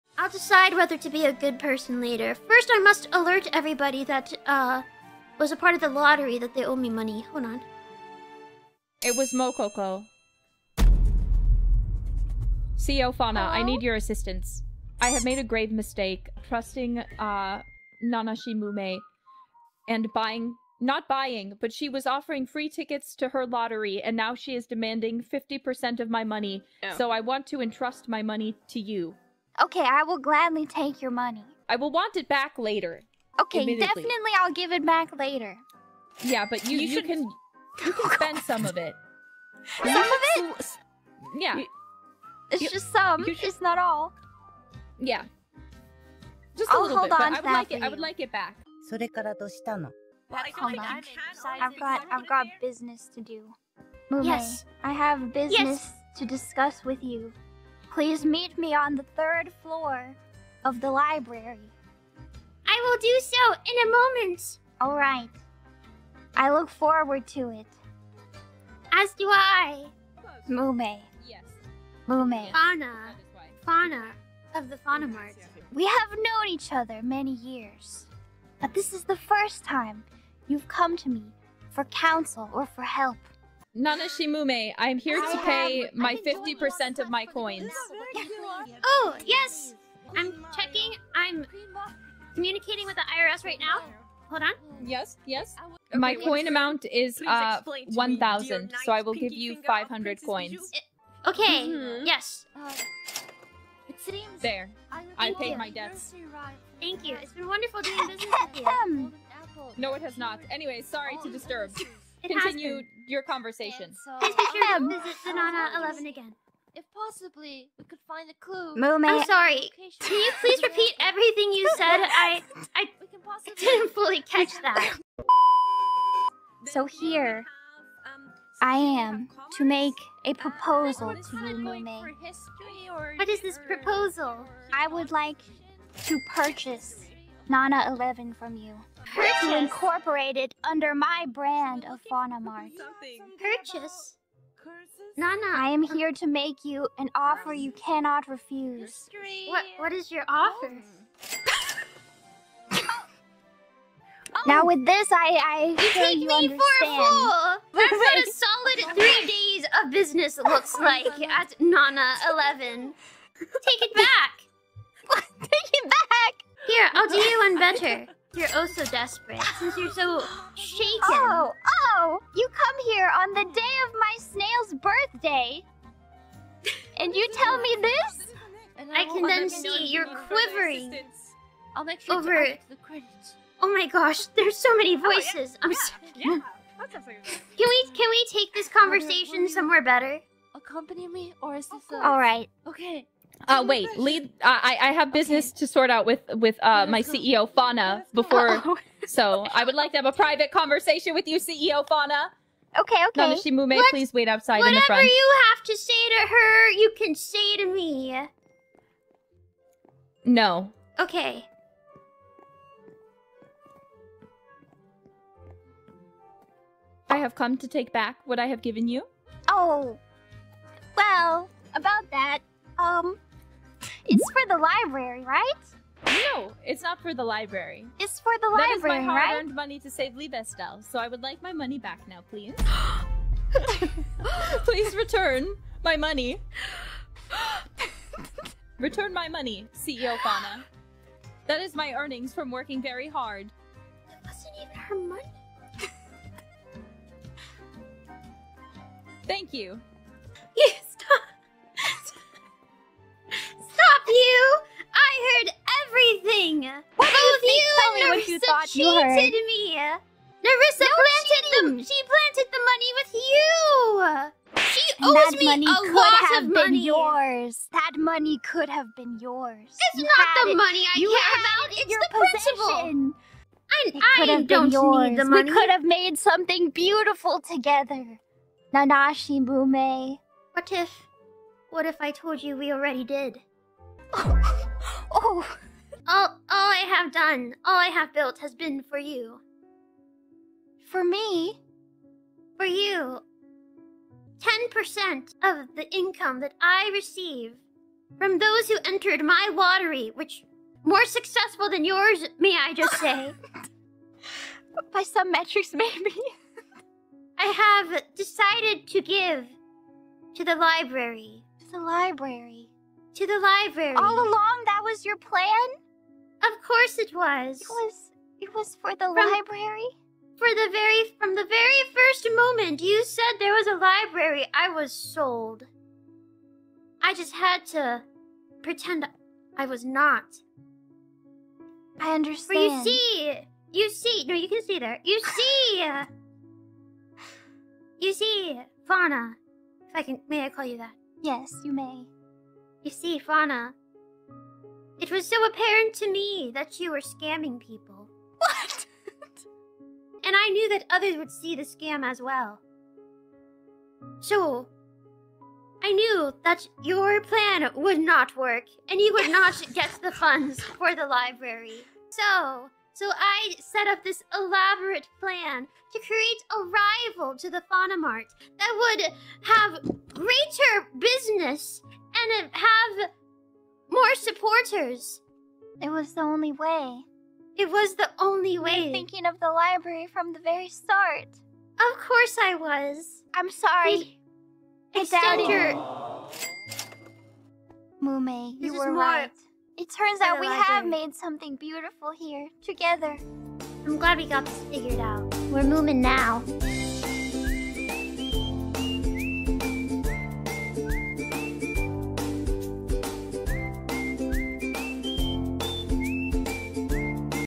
[ENG SUB/Hololive] The shocking truth behind Faunamart and Nana-Eleven rivalry
Credits :
BGM